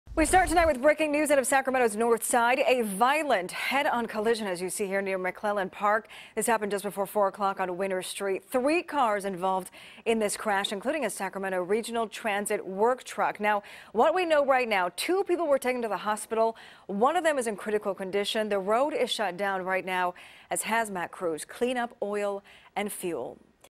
2 Injured In Violent Head-On Crash
Hazmat crews are on the scene of a violent head-on collision near McClellan Park.(3/28/19)